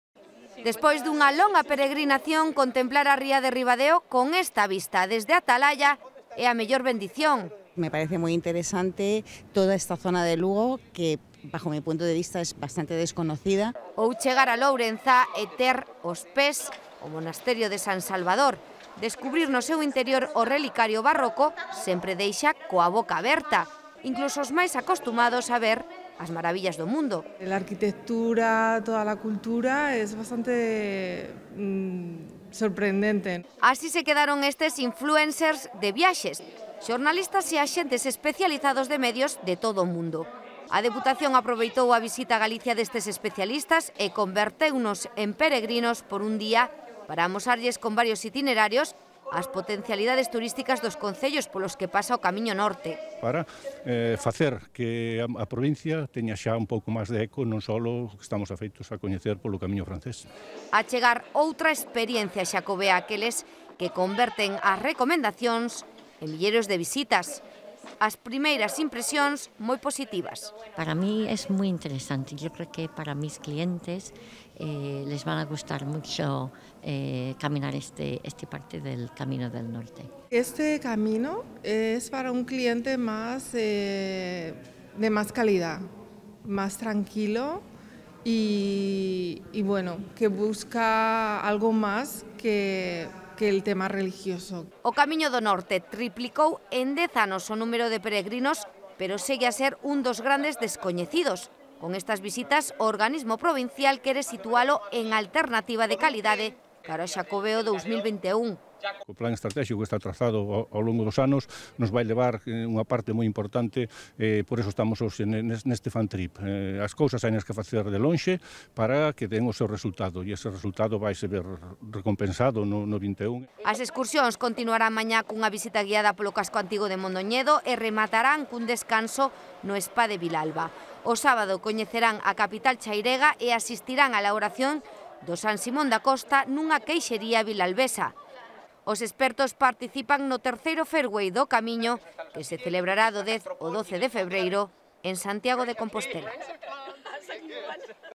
Expertos en turismo, de peregrinación na Mariña
Despois dunha longa peregrinación, contemplar a ría de Ribadeo, con esta vista, dende a Atalaia é a mellor bendición; ou chegar a Lourenzá e meter os pés no Mosteiro de San Savador, descubrir no seu interior o relicario gótico sempre deixa coa boca aberta.